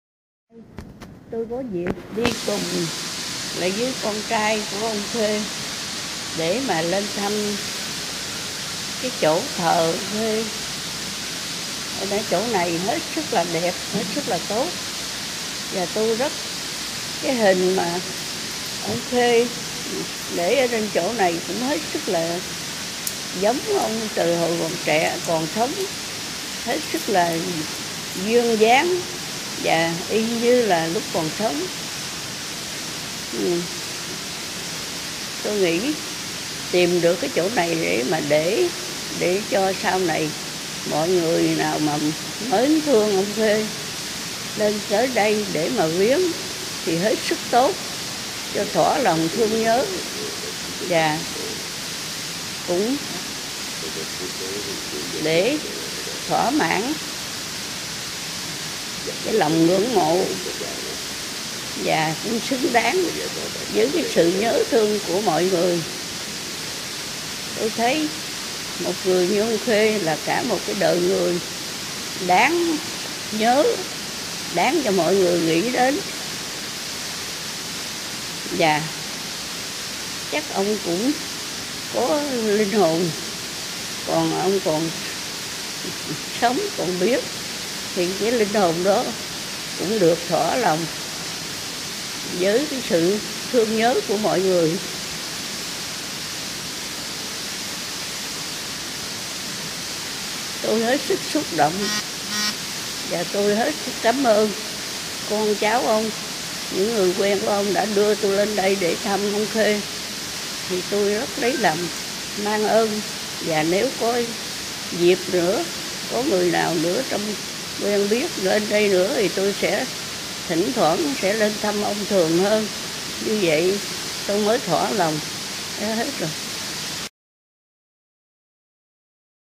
BÀ MAI THỊ TRÌNH VIẾNG CỐ GS TRẦN VĂN KHÊ
Chiều 16/1/2016, bà Mai Thị Trình, 91 tuổi, một trong những người bạn thân của cố GS Trần Văn Khê từ thời ở Paris, đã đi viếng và thắp nhang trước di cốt của giáo sư tại gian tưởng niệm ở Linh Hoa Tuệ Đàn, Hoa viên Nghĩa trang Bình Dương.
Bà Trình nguyên là người phụ trách bản tin tiếng Pháp của Thông tấn xã Việt Nam, từng là một trong những người bạn thân ở Paris của GS Trần Văn Khê. Theo GS Khê, bà Trình có tổng cộng tới ba năm luôn thăm ông, nghe ông kể chuyện bên giường bệnh vào những lúc ông phải nằm bệnh viện ở Pháp.